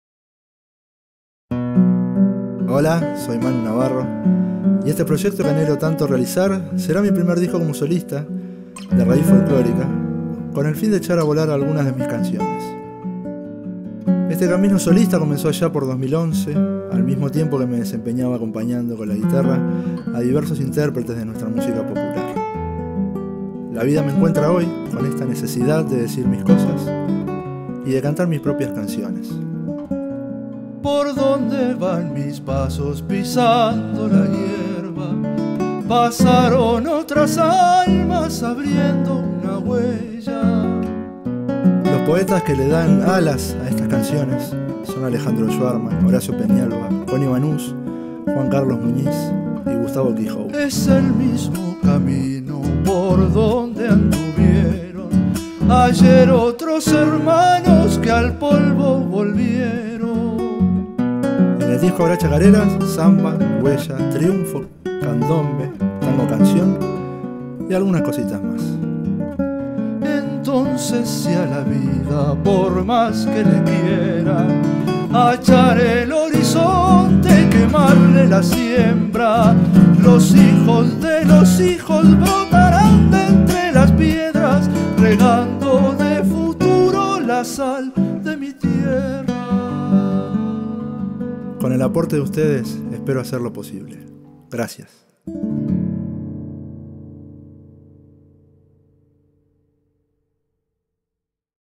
Manu Navarro - Mi primer disco solista - IdeaMe
Este proyecto que anhelo tanto realizar, será mi primer disco como solista, de raíz folklórica, con el fin de echar a volar algunas de mis canciones.
Este camino solista comenzó allá por 2011, al mismo tiempo que me desempeñaba acompañando con la guitarra a diversos intérpretes de nuestra música popular.
La vida me encuentra hoy con esta necesidad de decir mis cosas y de cantar mis propias canciones.
Los poetas que le dan alas a estas canciones son Alejandro Szwarcman, Horacio Peñalva, Coni Banús, Juan Carlos Muñíz y Gustavo Kehoe.
Habrá zamba, chacarera, huella, triunfo, candombe, tango-canción y otras cositas más.
Con el aporte de ustedes espero hacerlo posible.
¡¡Gracias, les dejo el link para que puedan aportar!!